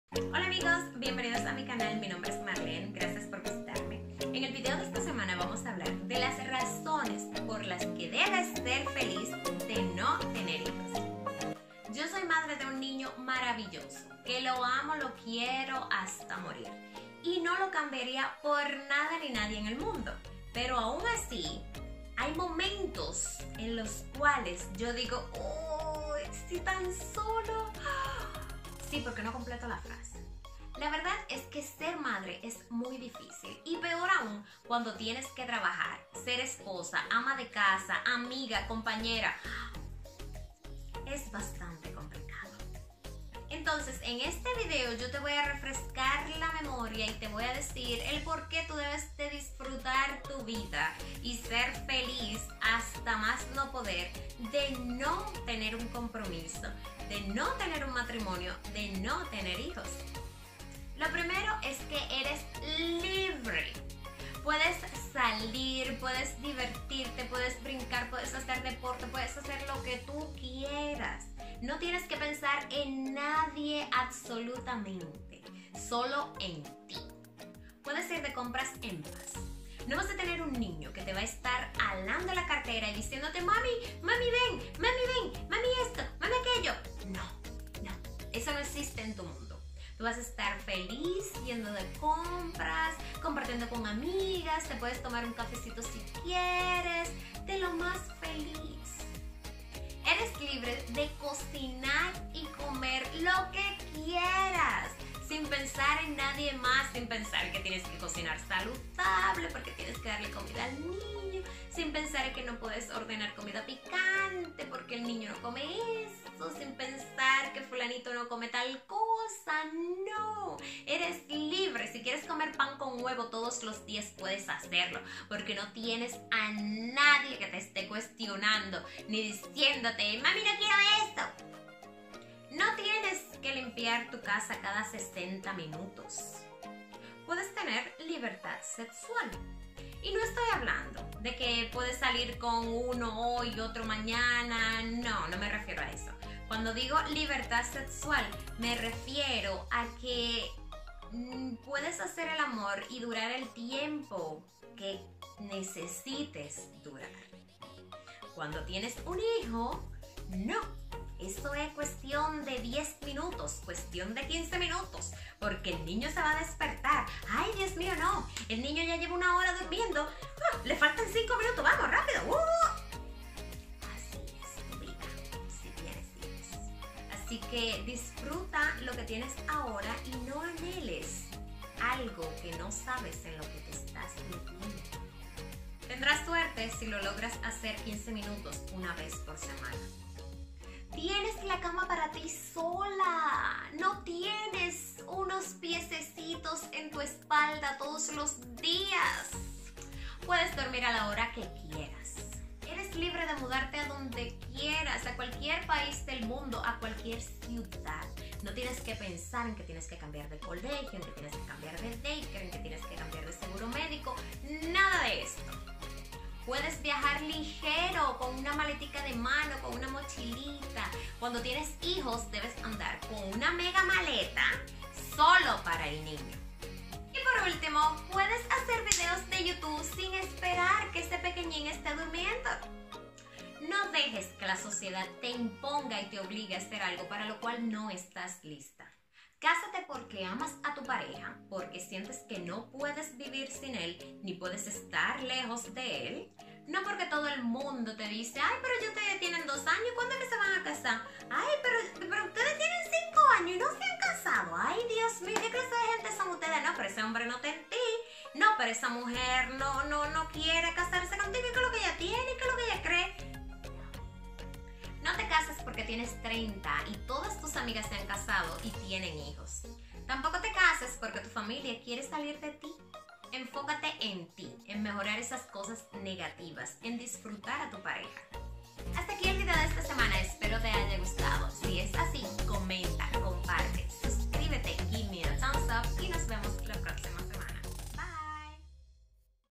10 Razones para NO tener hijos antes de estar lista👶 y haber disfrutado tu vida🤰🤱
En este video te daré 10 razones para que saques la idea de ser madre de tu cabeza hasta que realmente estes preparada para serlo.

Sin pensar en que tienes 30 años y que necesitas formar familia porque la sociedad te lo impone.